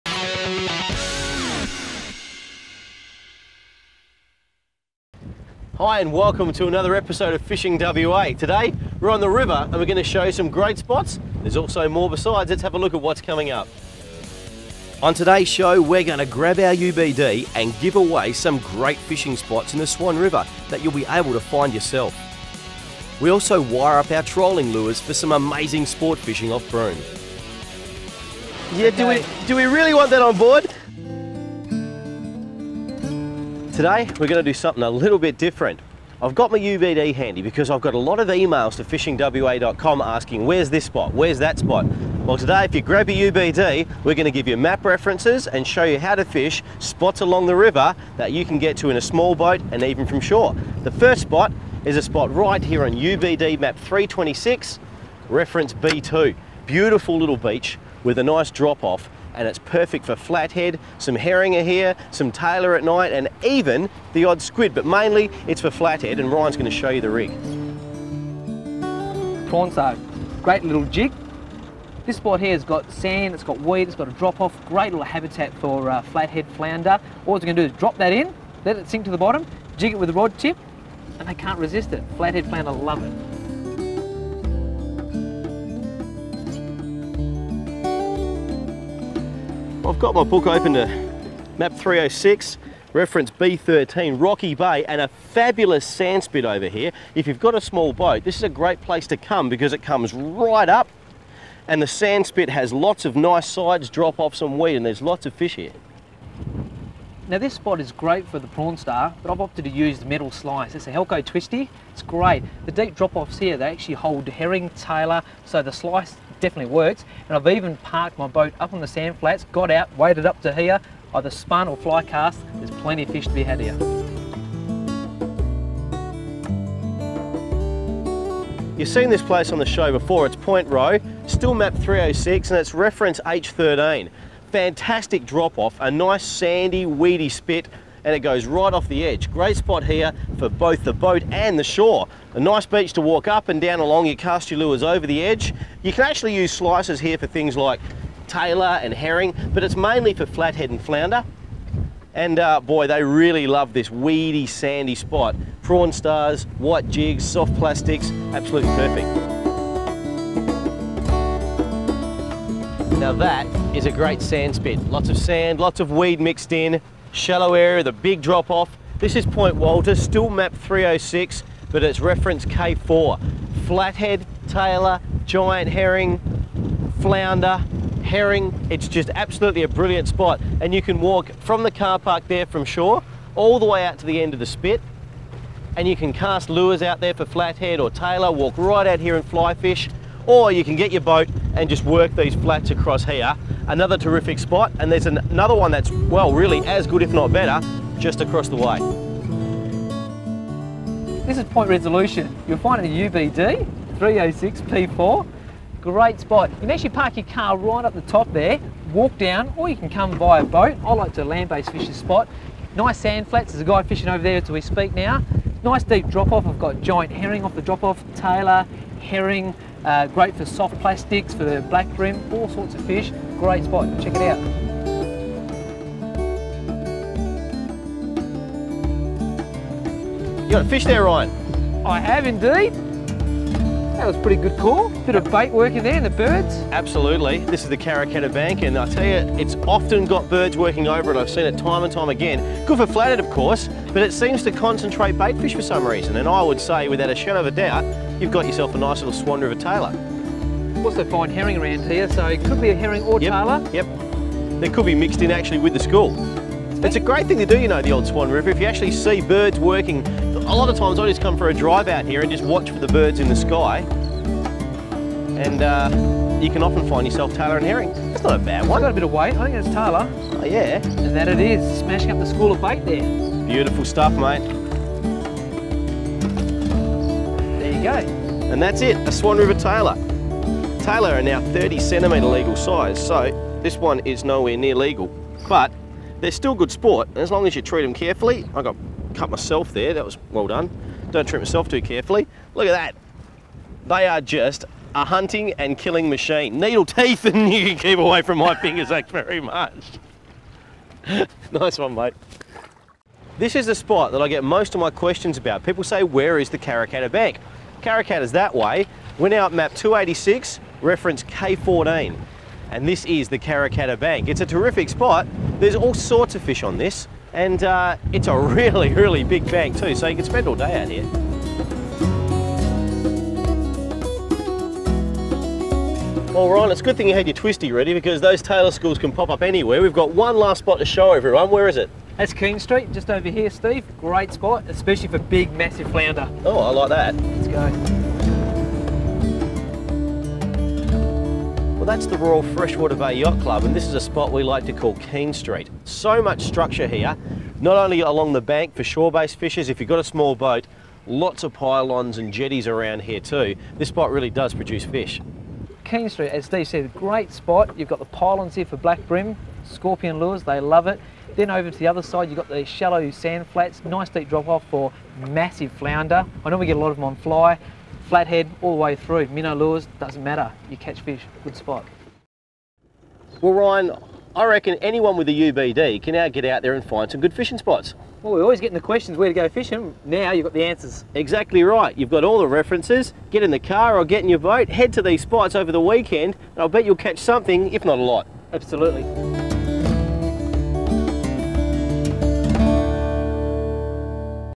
Series 5 Ep 15 Swan river secrets
Steve heads out on the swan river and shows you all his fishing spots